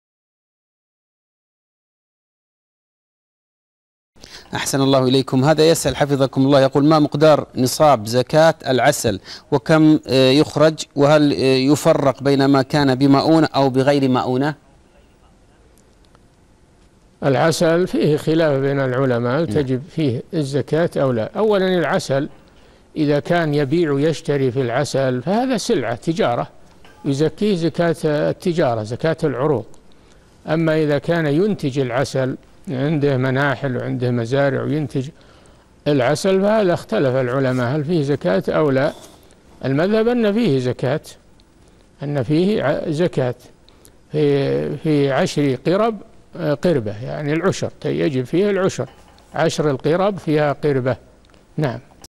حكم الزكاة في العسل - الشيخ صالح الفوزان
-نص السؤال: هذا يسأل حفظكم الله يقول: ما مقدار نصاب زكاة العسل، وكم يخرج، وهل يفرّق بين ما كان بمئونة أو بغير مئونة؟
للاشتراك في قناة الشيخ صالح الفوزان

للاستفسار والإقتراحات والدعم
رئيس الفريق
لمشروع كبار العلماء